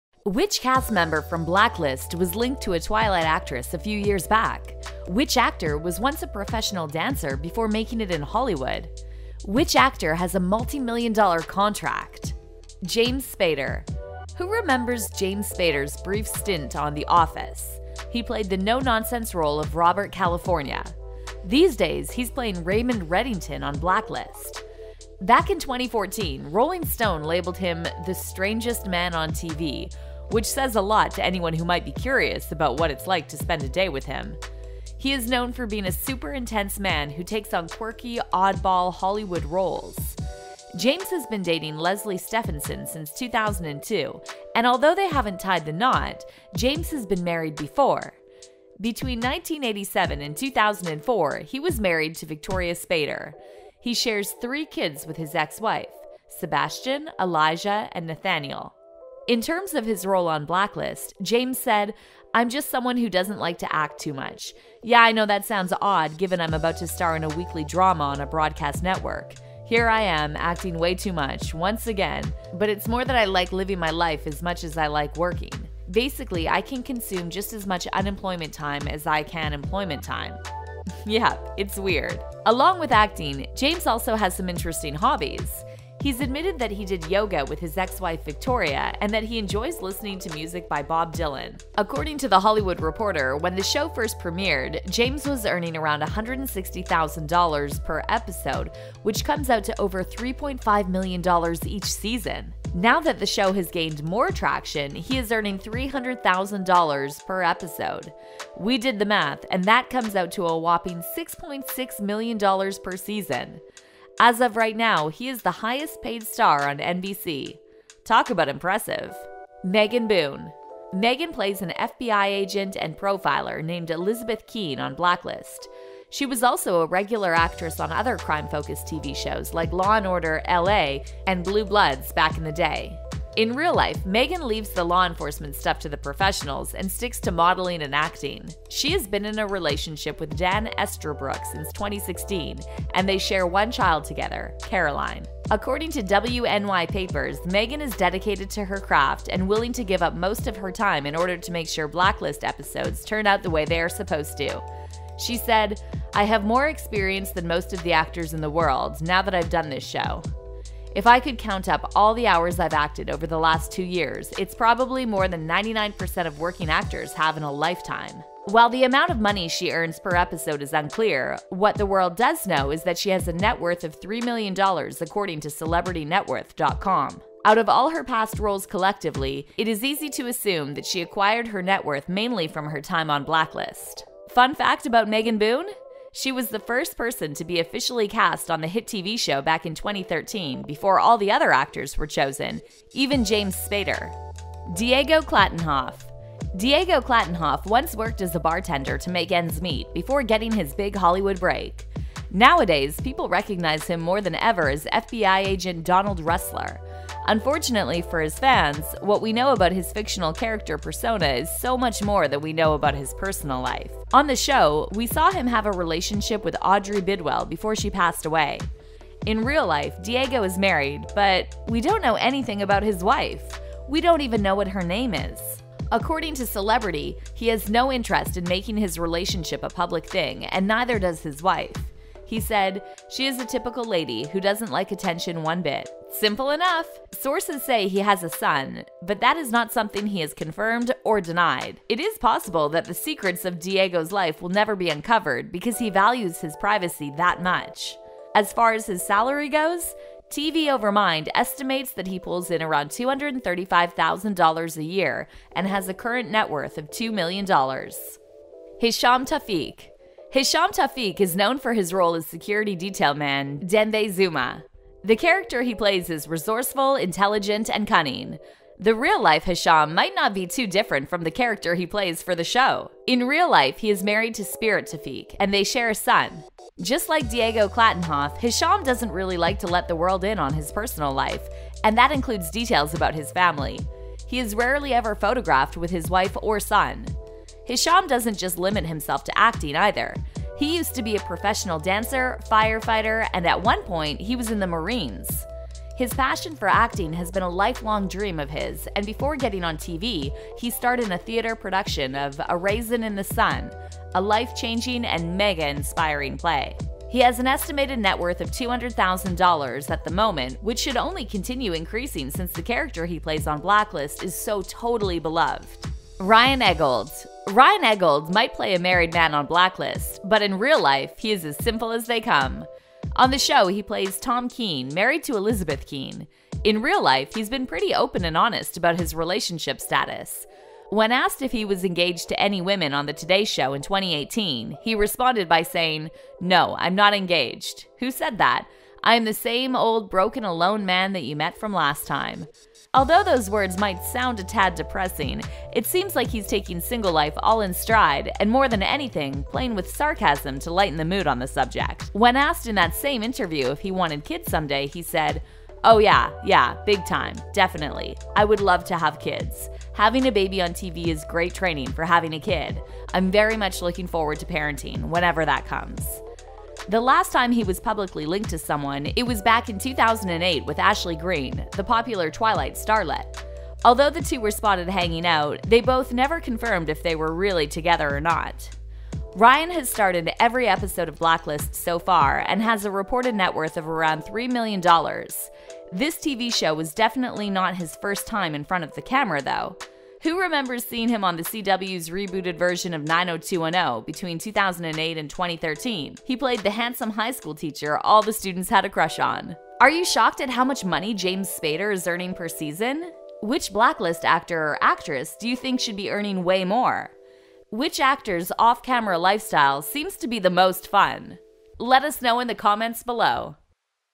The Blacklist: Real Lifestyles, Couples, Salaries Revealed 2020
Which cast member from Blacklist was linked to a Twilight actress a few years back? Which actor was once a professional dancer before making it in Hollywood? Which actor has a multi-million-dollar contract? This is The Blacklist cast real lifestyles revealed!

Wether you're looking for netflix shows to binge watch or trying to stay updated with the latest tiktok stars, we got it covered. Tune in to see everything pop culture related!